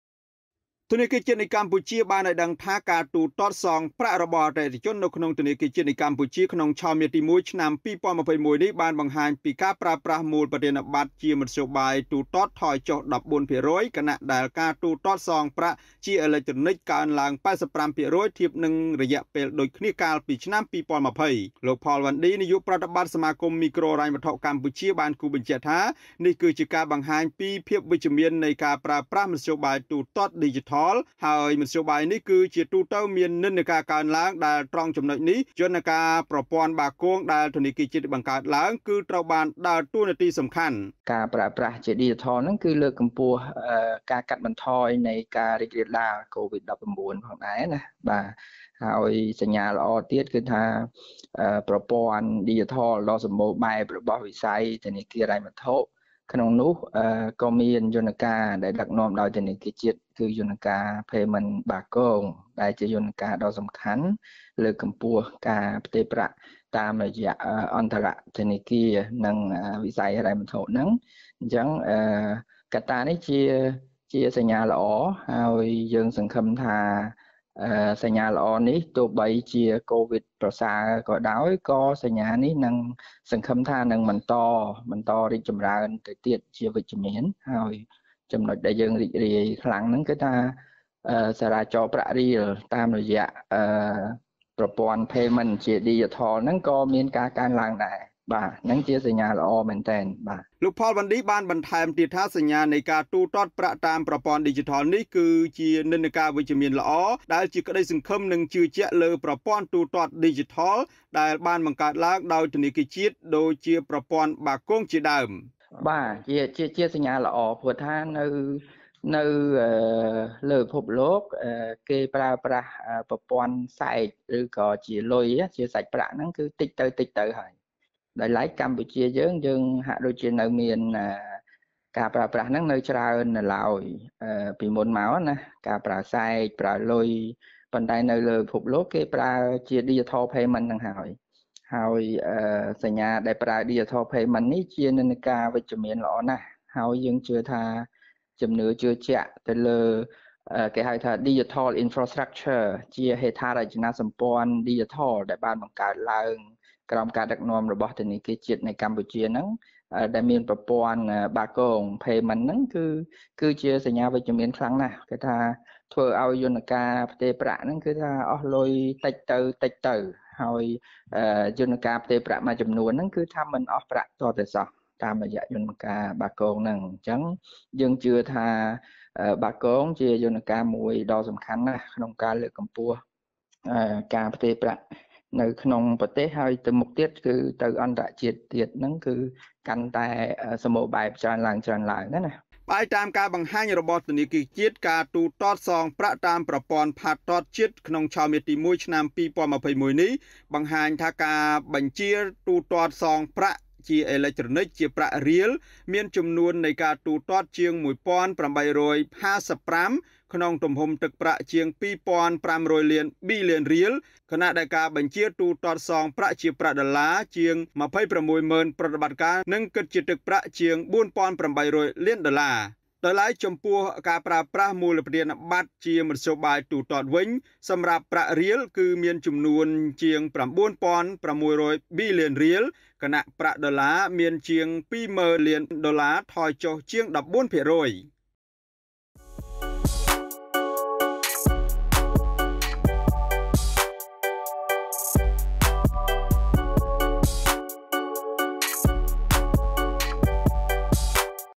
[07-12-21]ធនាគារជាតិថា ការទូទាត់សងប្រាក់ជាអេឡិកត្រូនិក កើនឡើង ៨៥% ខណៈការទូទាត់មូលប្បទានបត្រថយចុះ ១៤%
ធនាគារជាតិថា ការទូទាត់សងប្រាក់ជាអេឡិកត្រូនិក កើនឡើង ៨៥% ខណៈការទូទាត់មូលប្បទានបត្រថយចុះ ១៤ភាគរយ